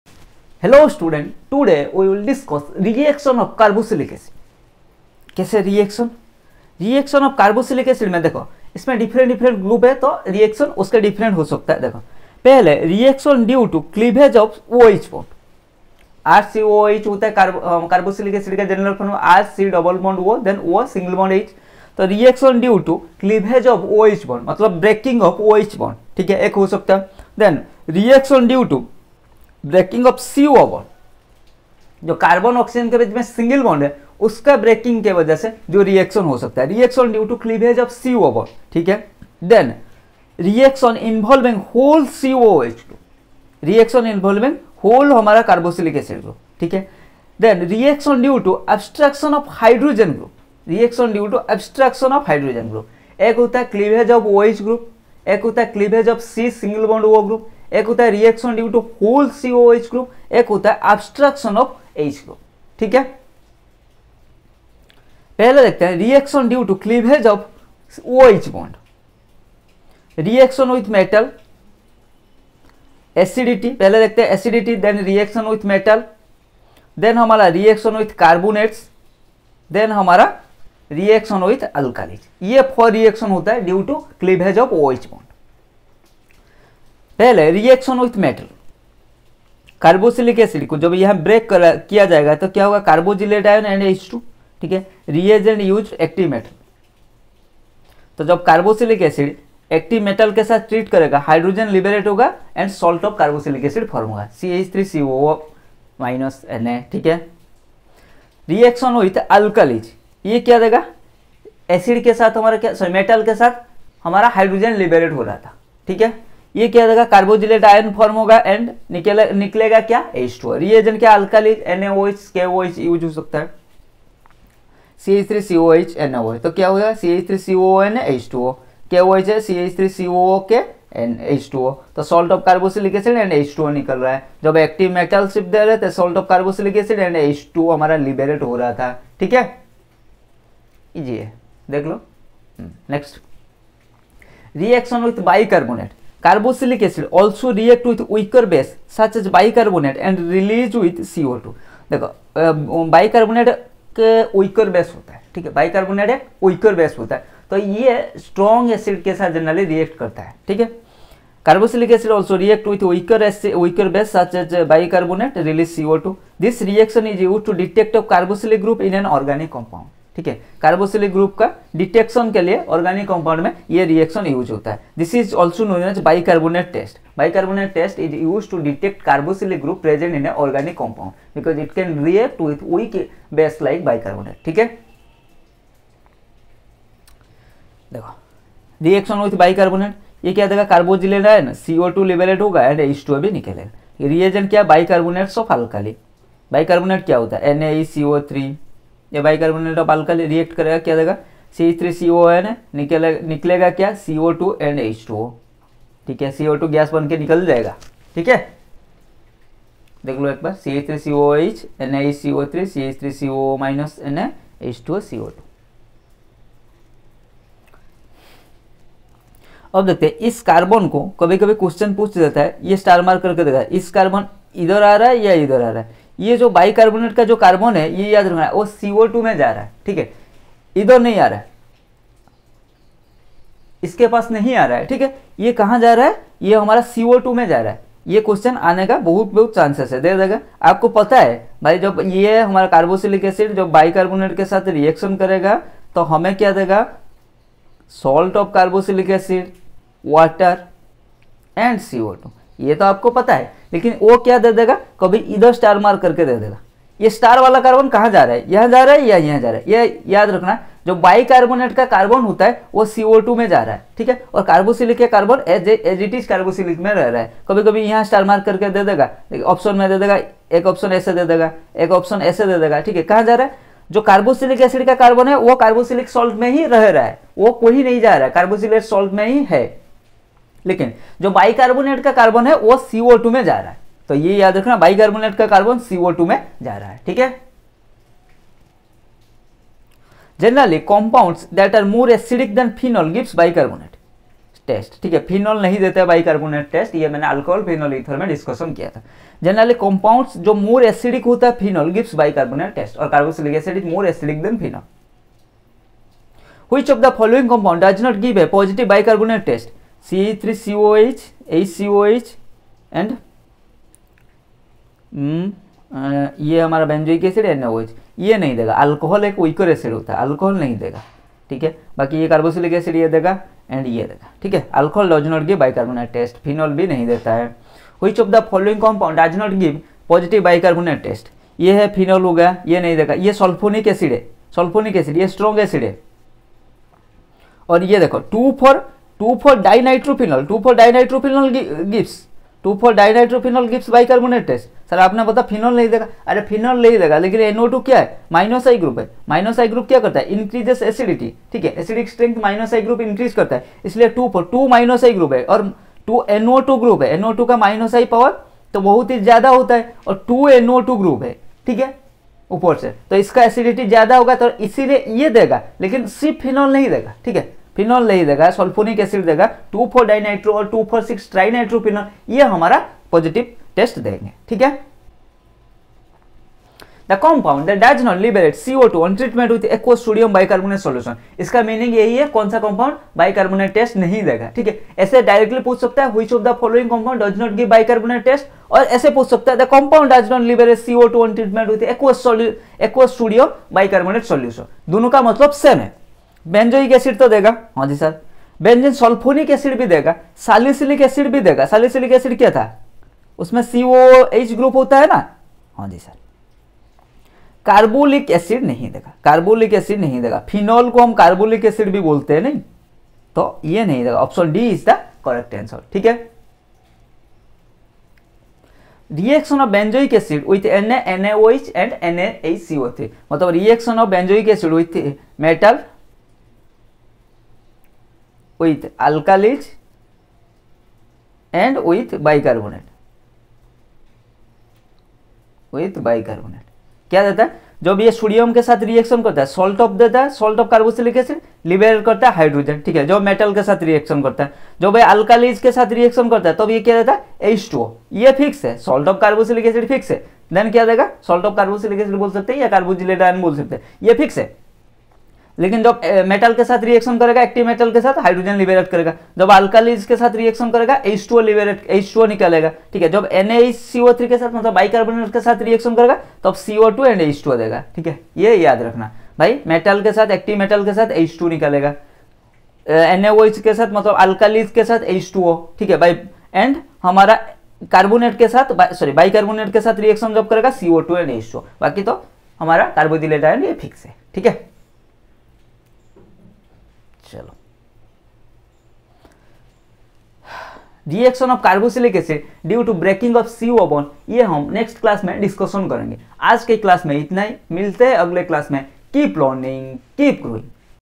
Carboxylic acid L5 ll Reaction due to cleavage of OH bond ll JEE ll NEET ll Mandal sir
Reaction of carboxylic acid
Reaction due to cleavage of OH bond
Reaction with Na metal
Reaction with sodium bicarbonate
Chemistry for jee
Chemistry for NEET
Chemistry for jee advanced
Chemistry for jeemain
Playlist for class 12th